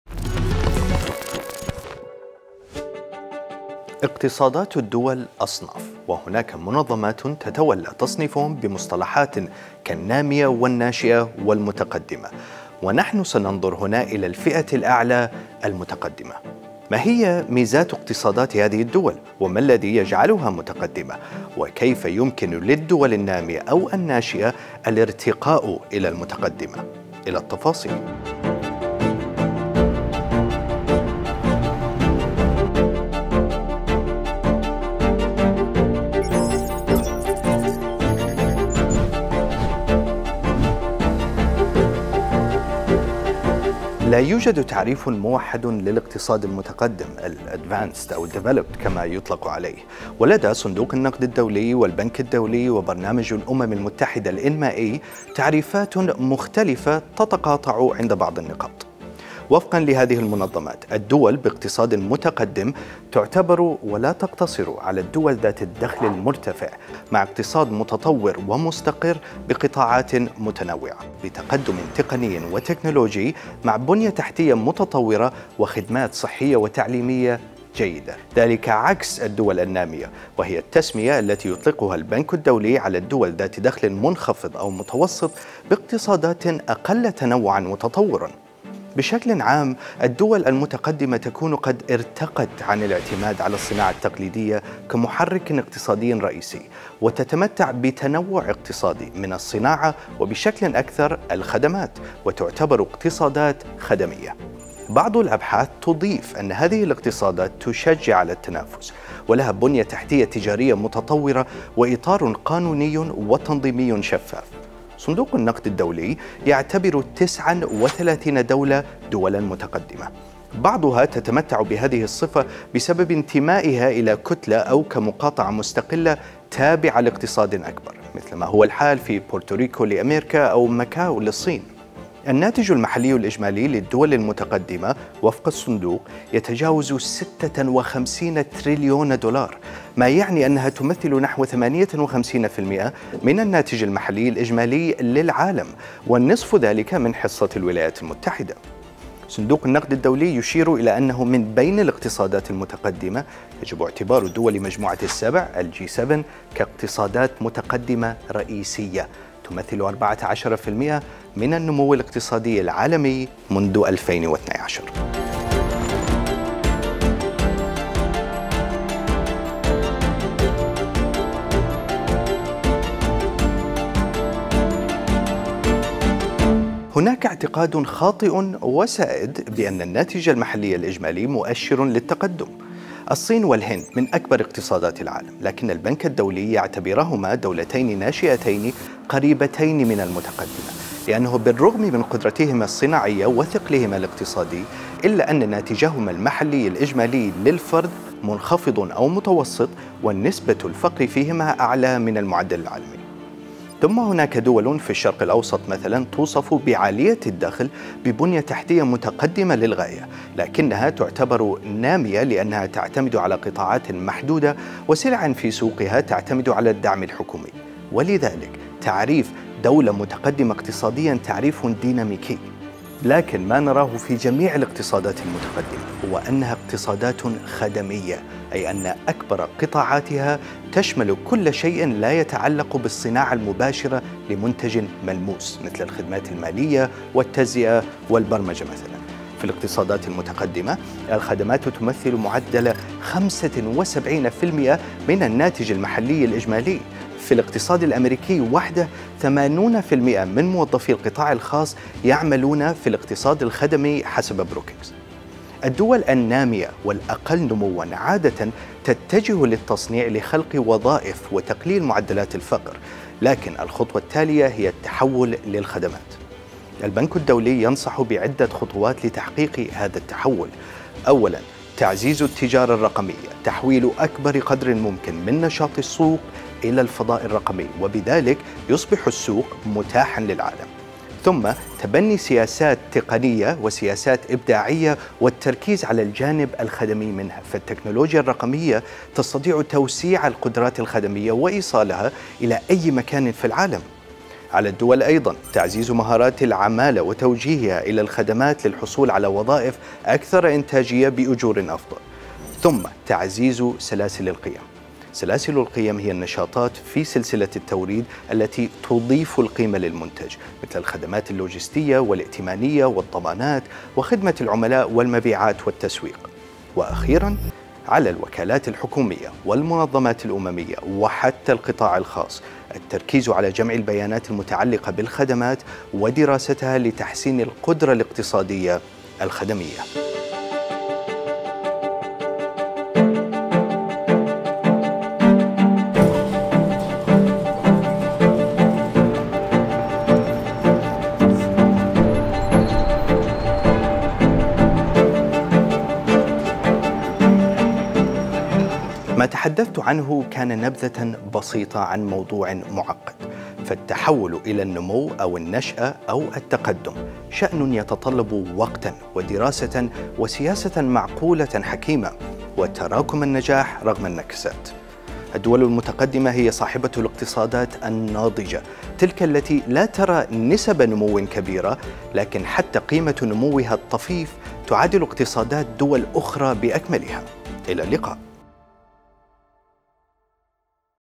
كيف تصبح الدول "اقتصادات متقدمة"؟ | أبعاد مع أرفد
: الاقتصادات المتقدمة تعتبر تلك المتطورة والمكونة من مزيج من القطاعات الهامة لكن مع ذلك لا يوجد تعريف موحد للاقتصاد المتقدم والتصنيف بحد ذاته ديناميكي. فما هي الاقتصادات المتقدمة وكيف تكّون؟